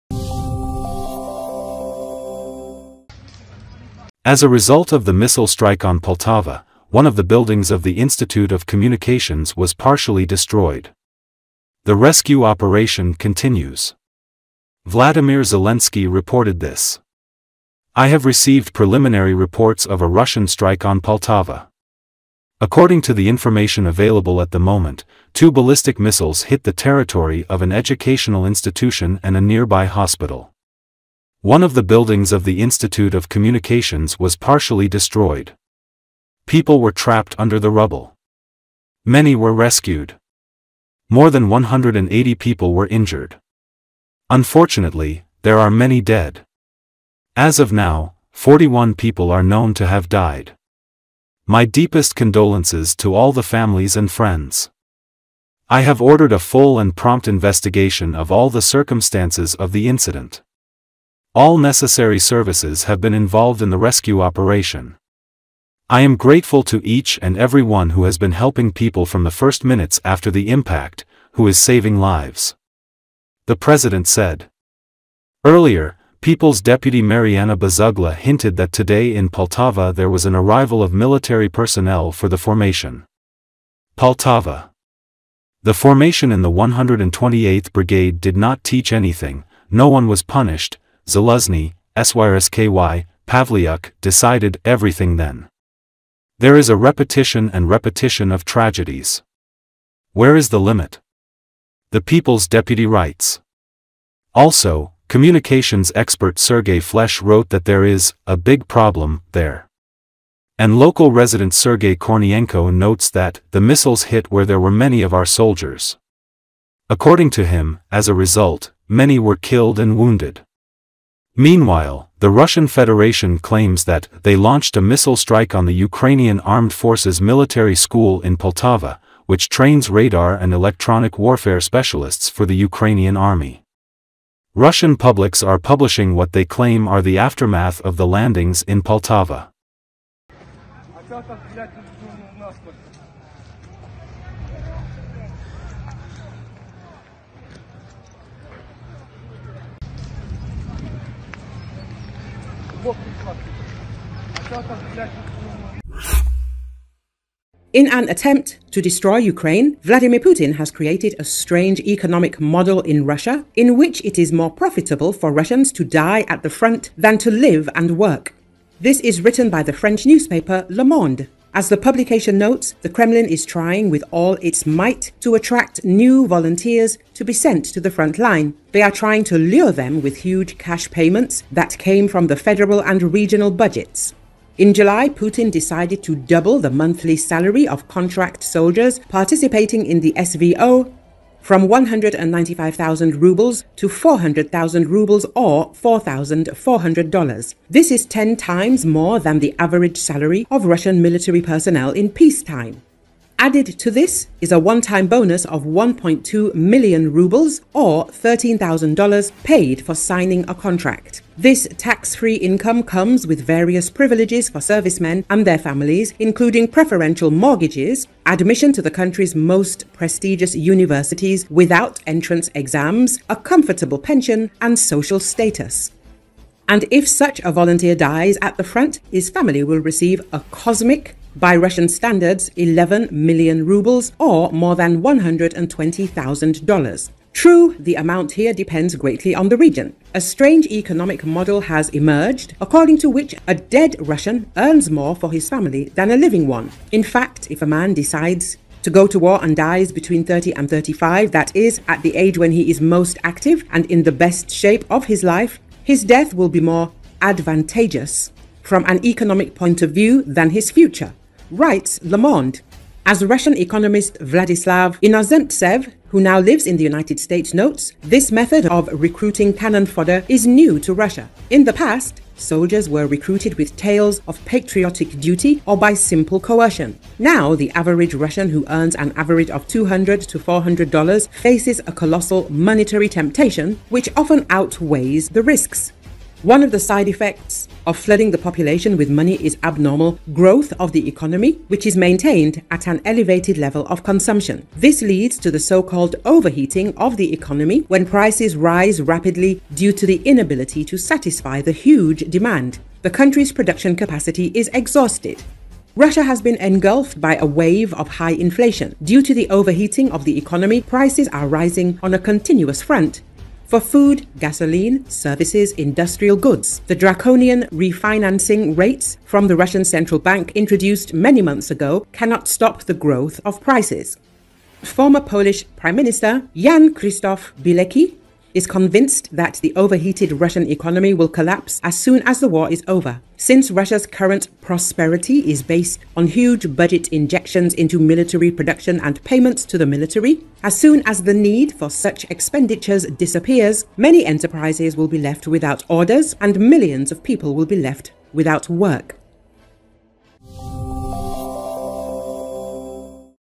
Russia hit a communications institute with hundreds of people, 41 dead, more than a hundred injured
As a result of the missile strike on Poltava, one of the buildings of the Institute of Communications was partially destroyed. The rescue operation continues. Vladimir Zelensky reported this .
"I have received preliminary reports of a Russian strike on Poltava. According to the information available at the moment, two ballistic missiles hit the territory of an educational institution and a nearby hospital.
One of the buildings of the Institute of Communications was partially destroyed. People were trapped under the rubble. Many were rescued. More than 180 people were injured. Unfortunately, there are many dead.
As of now, 41 people are known to have died. My deepest condolences to all the families and friends.
I have ordered a full and prompt investigation of all the circumstances of the incident. All necessary services have been involved in the rescue operation. I am grateful to each and every one who has been helping people from the first minutes after the impact, who is saving lives!" the president said.

Earlier, People's Deputy Maryana Bezugla hinted that today in Poltava there was an arrival of military personnel for the formation.

"Poltava. The formation in the 128th brigade did not teach anything, no one was punished, Zaluzhny, Syrsky, Pavlyuk "decided" everything then. There is a repetition and repetition of tragedies. Where is the limit?" - the people's deputy writes.

Also, communications expert Sergei Flesh wrote that there is “a big problem” there.

And local resident Sergei Kornienko notes that "the missiles hit where there were many of our soldiers." According to him, as a result, "many were killed and wounded."

Meanwhile, the Russian Federation claims that "they launched a missile strike on the Ukrainian Armed Forces military school in Poltava, which trains radar and electronic warfare specialists for the Ukrainian army." Russian publics are publishing what they claim are the aftermath of the landings in Poltava.